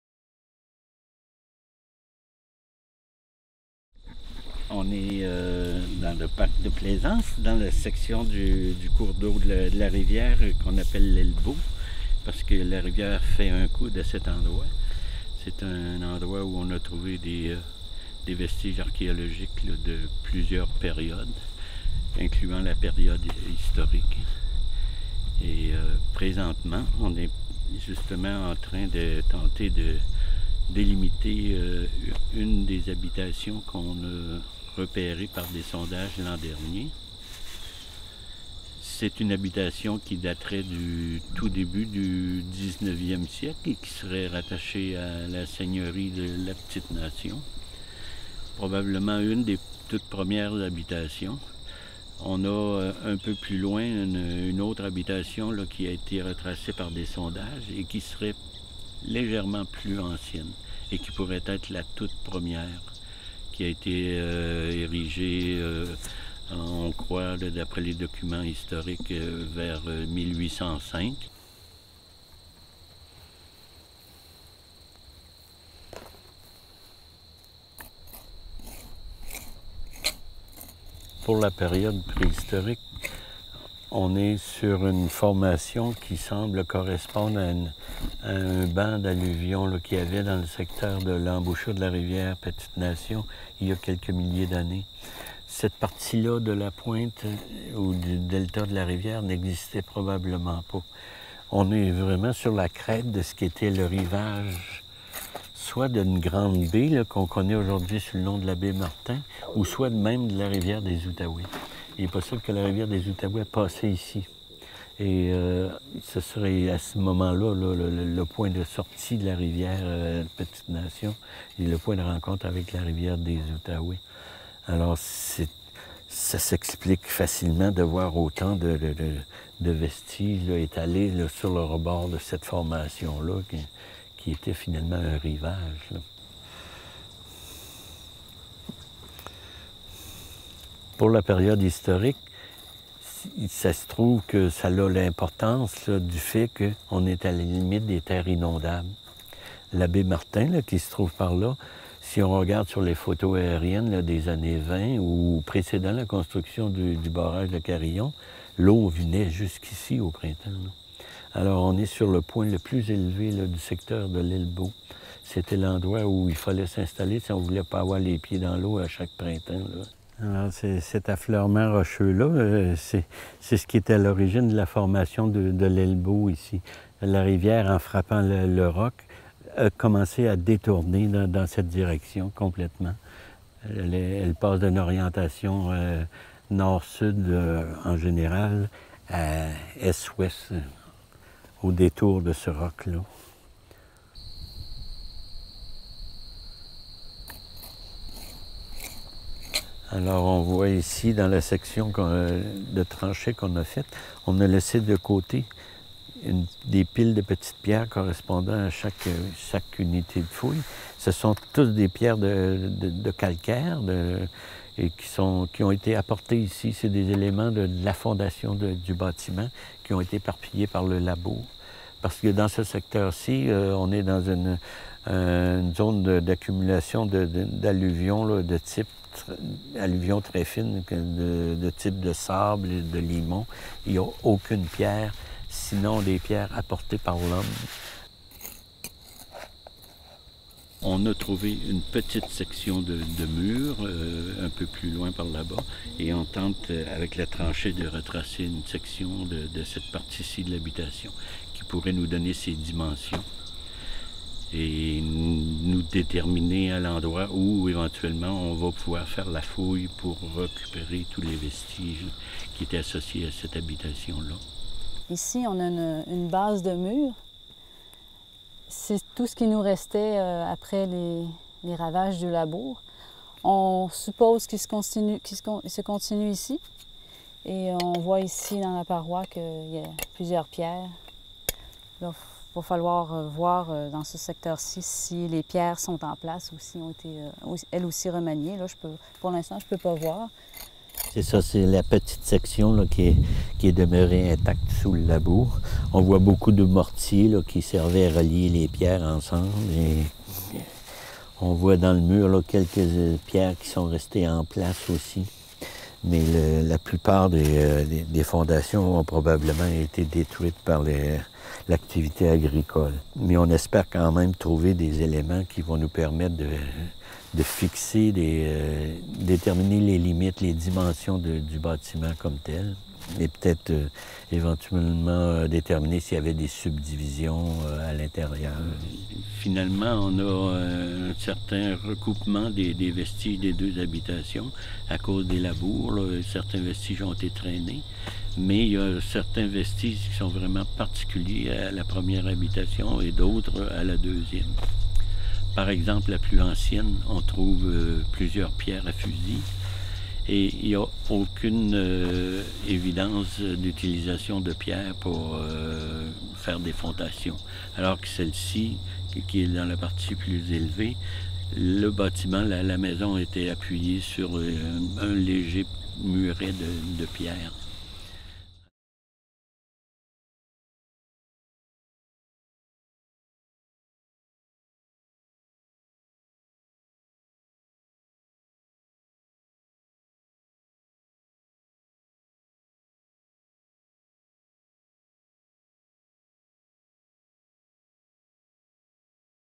Habiter la Petite-Nation
Dans le secteur fouillé sous la gouverne du GRAO pour le parc national de Plaisance, l’équipe a mis au jour des vestiges de deux habitations. Une des habitations, la première du site, daterait de 1805 selon les archives. L’autre habitation est plus récente, une vingtaine d’années plus tard. On appelle ce secteur l’Elbow (pour coude). La rivière a frappé le roc et a été déviée ; s’écoulant nord-sud, elle passe en est-ouest pour contourner l’affleurement rocheux. Les photographies aériennes nous présentent le site avant le barrage Carillon au début des années 1960. Ces images ont permis aux archéologues de projeter le paysage alors que la rivière de la Petite Nation rencontrait la rivière des Outaouais près de cet endroit. Un endroit propice aux échanges, marqué par la présence de matériel préhistorique.